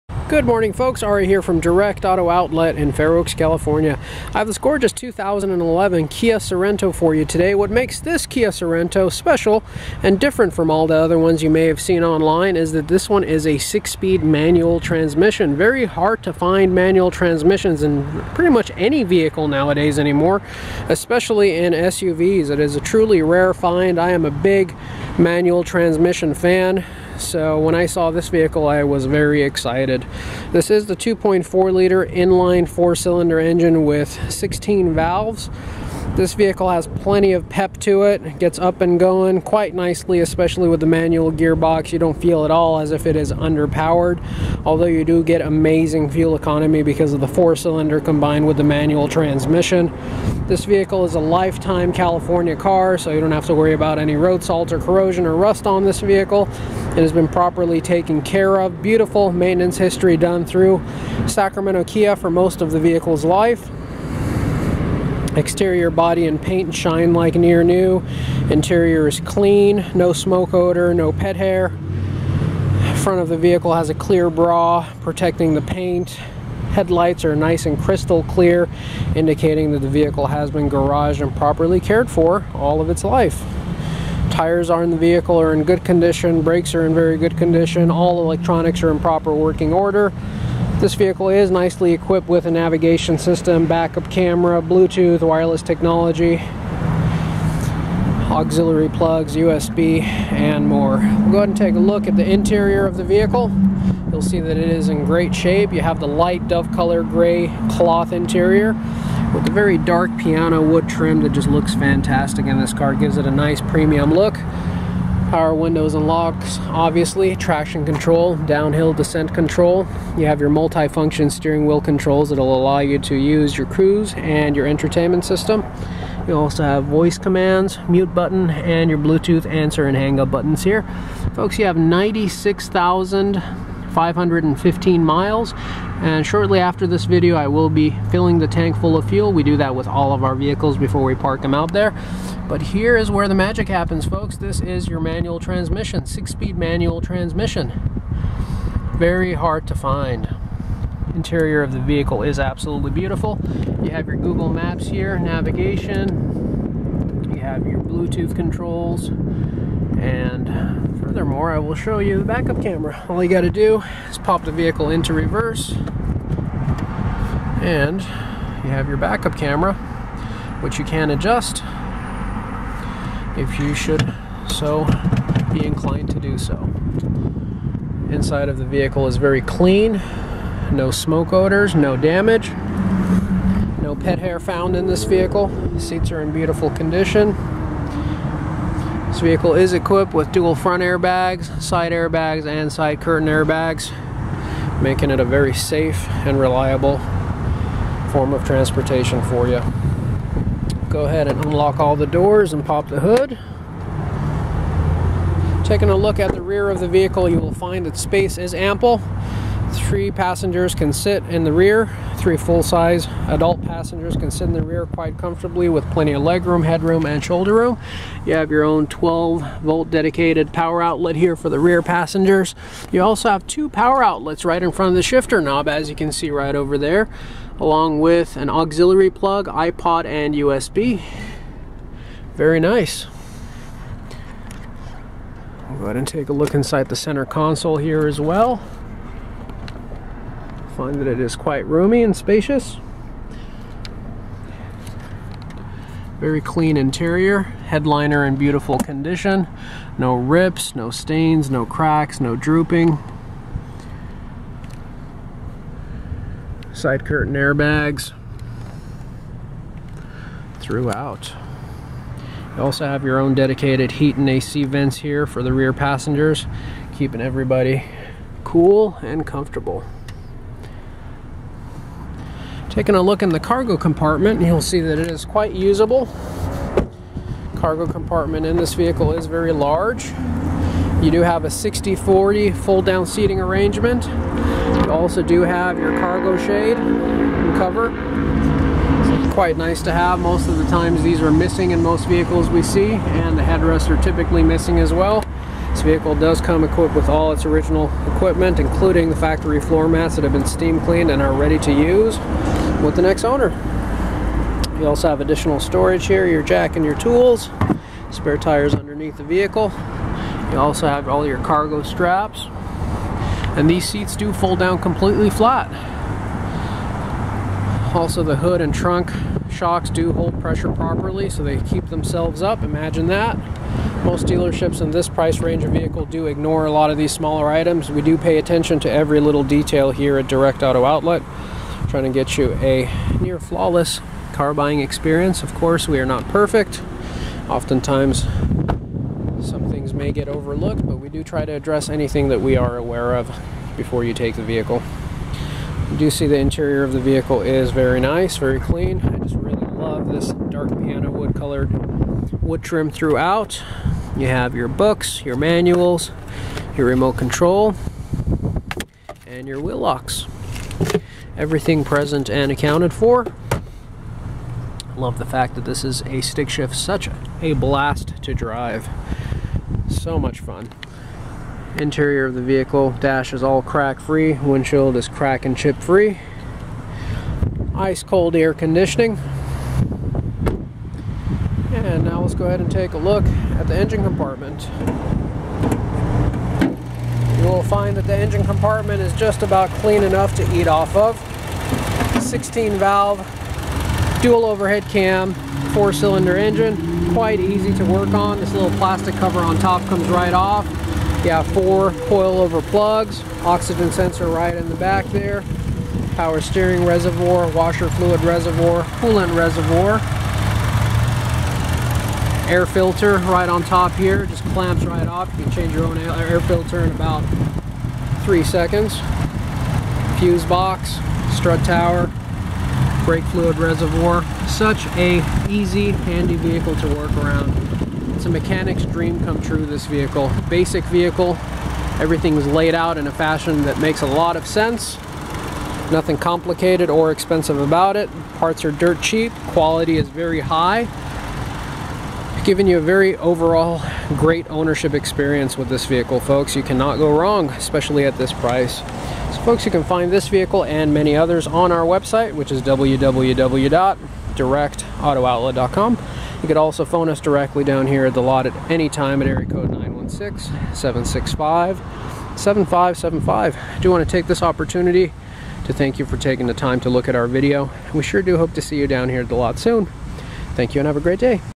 2011 Kia Sorento 6 speed manual transmission video overview and walk around.
Let the camera be your eyes and ears as we take you for a tour of this hard to find 6 speed manual Kia Sorento.  We will demonstrate the features and you will see and hear everything as if you were here in person.